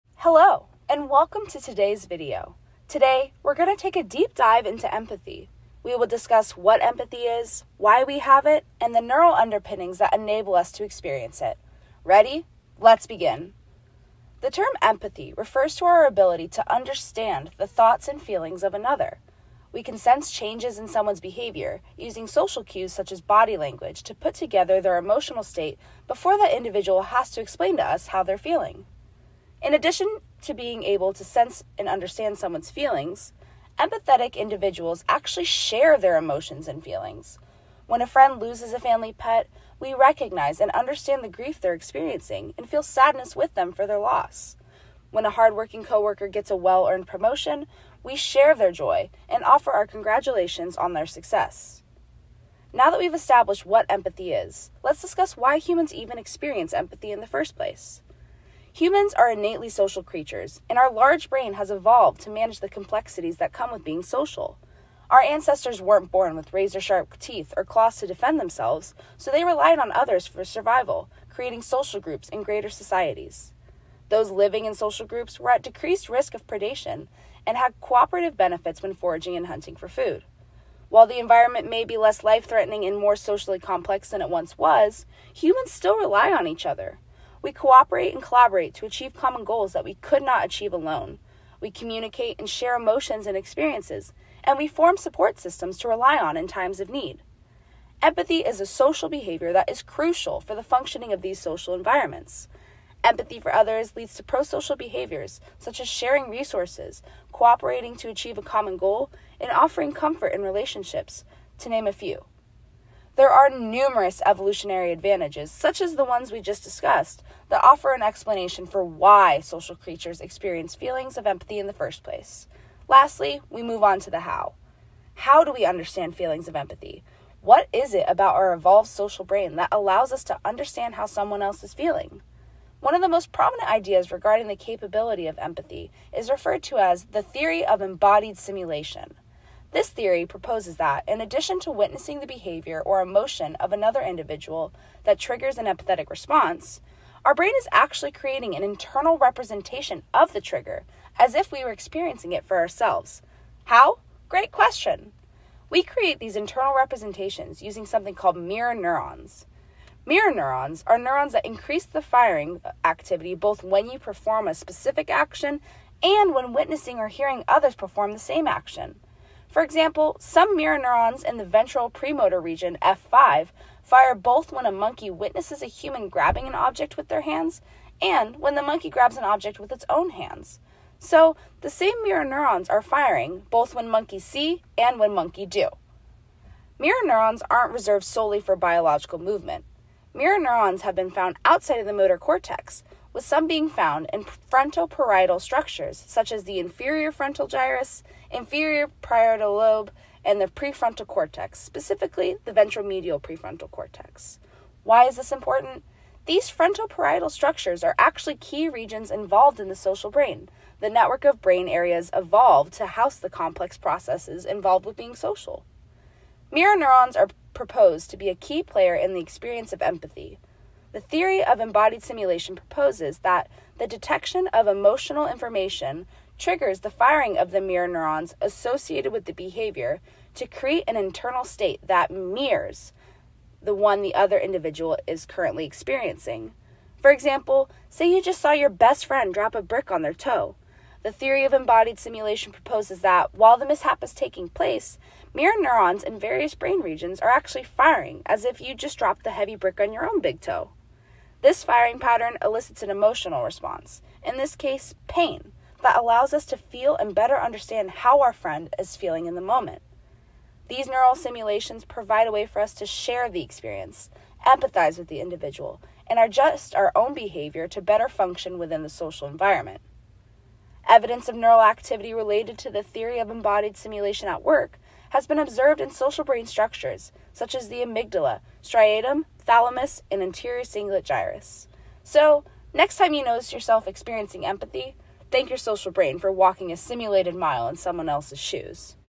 Empathy Mirror Neurons and the Theory of Embodied Simulation
Humans are innately social creatures, and the human brain evolved to employ empathy for survival benefits and to navigate society. This human capacity for empathy is largely attributed to the theory of embodied simulation, which proposes that witnessing another individual's behavior or emotion triggers an empathetic response in our brain, which reacts as if we were experiencing it ourselves. This is enabled by specialized mirror neurons, which similarly fire both when we drop a brick on our toe and when we see someone else drop a brick on their own toe.

Created by Zoey Joshlin